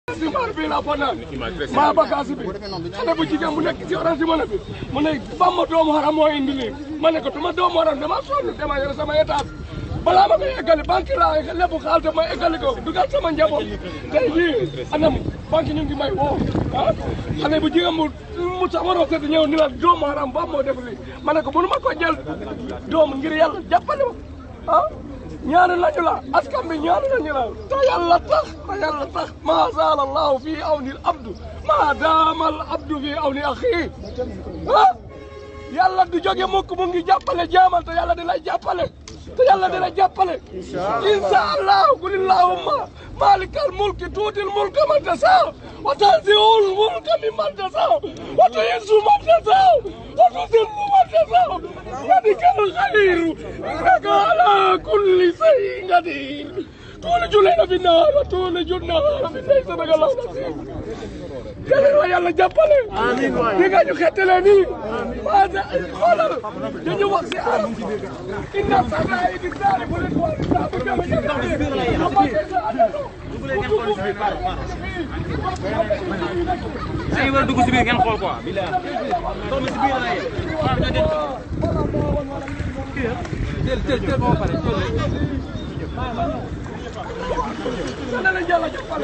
Visite de Sonko dans la banlieue : Émotion et tristesse au rendez-vous.
Xalima Tv vous offre des reportages, des interviews en exclusivite et des émissions originales. Retrouvez toutes nos vidéos sur les sites de et